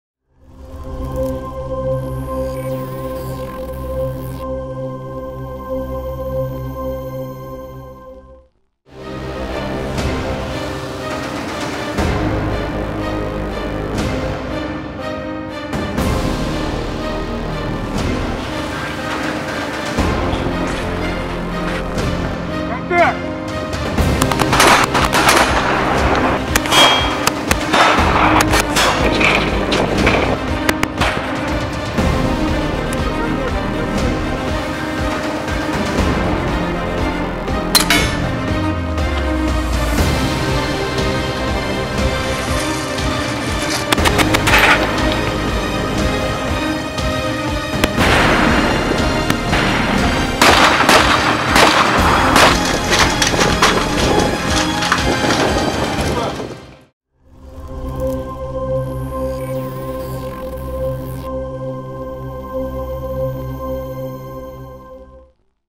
Тренування бійців ССО
Щодня як стиснута пружина

Сили спеціальних операцій Збройних сил України представлені в
Об'єднаних силах добре підготовленими окремими підрозділами. Завдання, до виконання яких готуються воїни ССО, мають найвищий ступінь складності. Тому щодня тривають тренування на землі, в повітрі, на воді та під водою. Та коли буде отримано команду на виконання бойового завдання, підрозділ Сил спеціальних операцій не залишить ворогові шансу.
Рухаємось далі!
Разом переможемо!
Слава Україні!